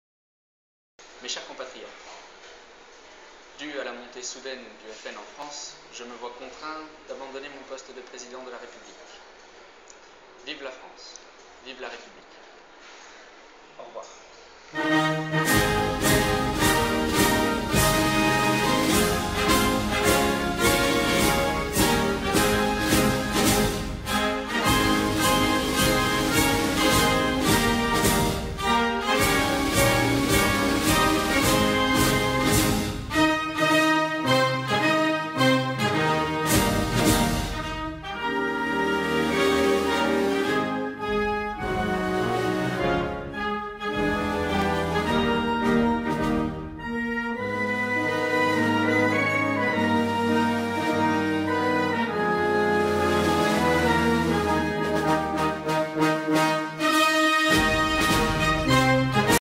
Les ravages du FN : le sacrifice d'un Homme dorénavant héros national
Funeste jour pour la République.En ce maudit 25 avril 2014, suite au séisme politique causé par la victoire du FN, le Président Samuel C. D'Estaing, renonce à ses fonctions.